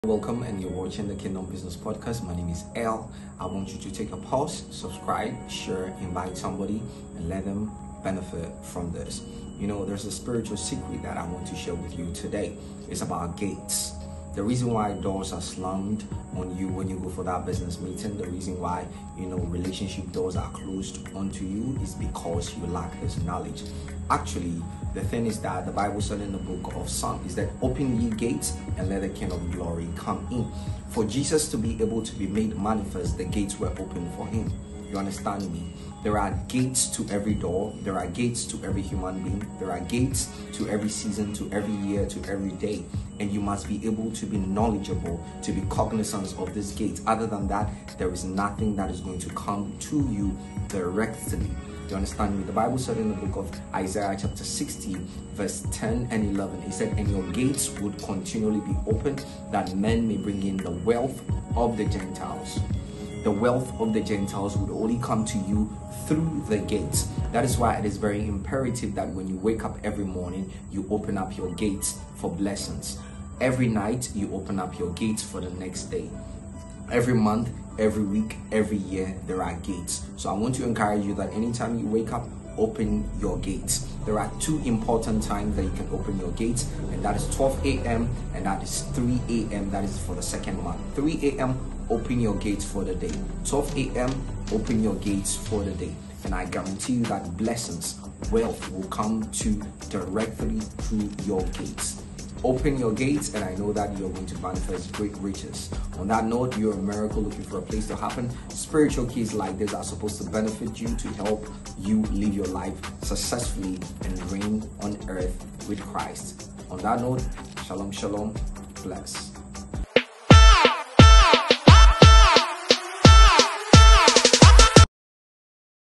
Truth About Spiritual Gates That Would Make You Successful In Life | Kingdombusiness season 2 Ep3
Spiritual truths about Gates that will aid you to be successful in life.

@elmenso Snr on all social media platforms.